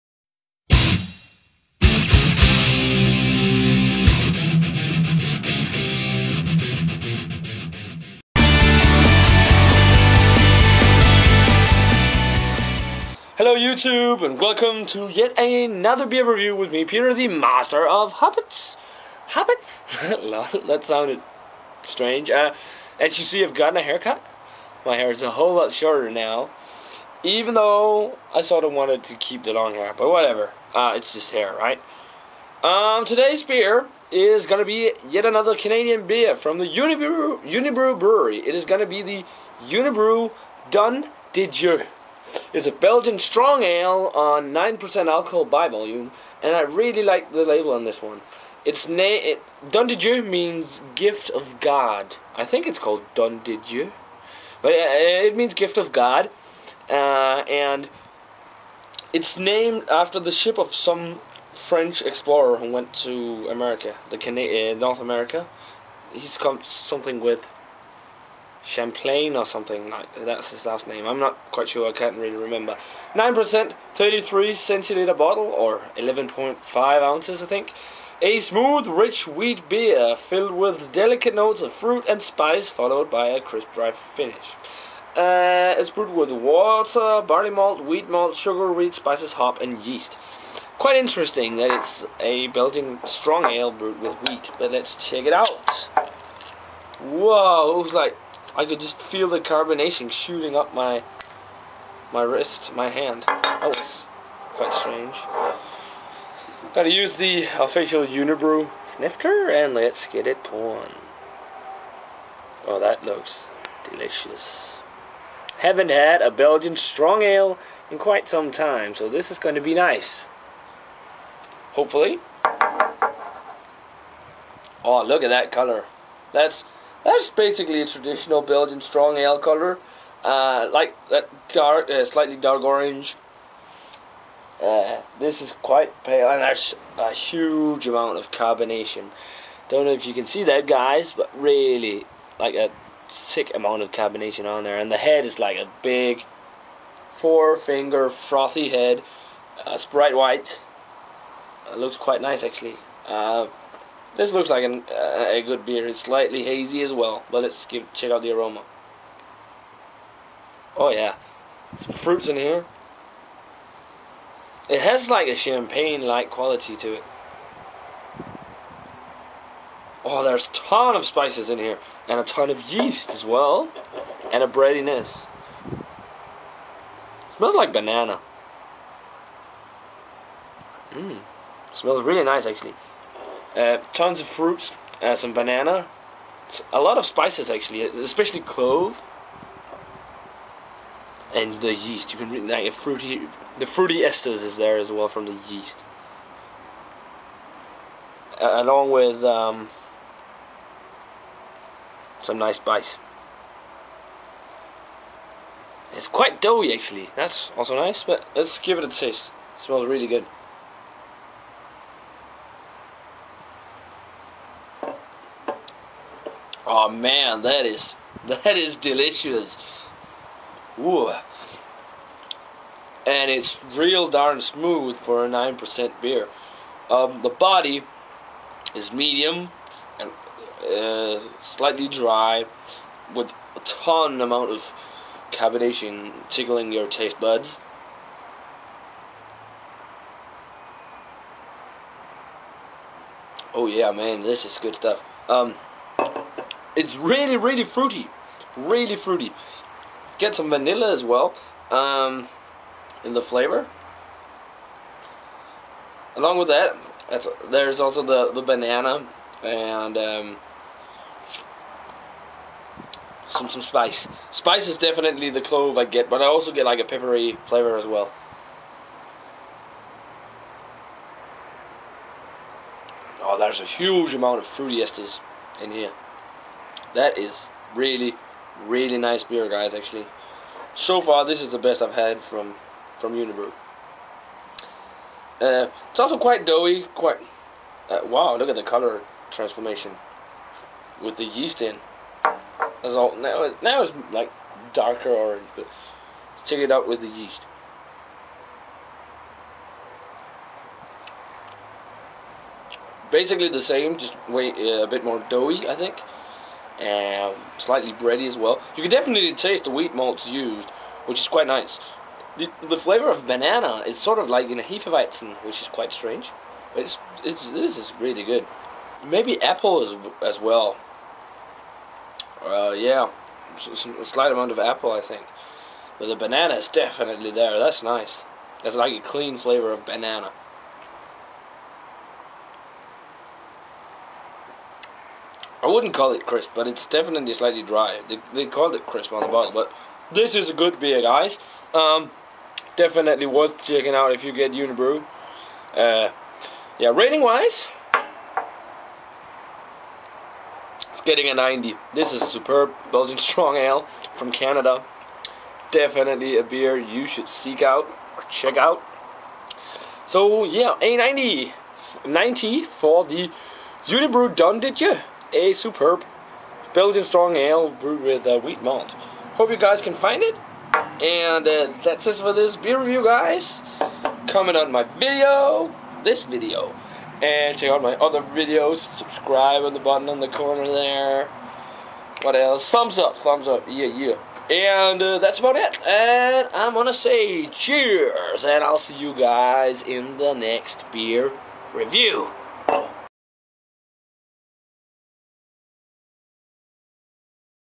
TMOH - Beer Review 173#: Unibroue Don de Dieu
Information About This Beer:

Brewery: Unibroue
Beer: Unibroue Don de Dieu
Style: Belgian Strong Ale
Location: Brewed by in Chambly, Quebec, Canada.
ABV: 9%
Rating: 90/100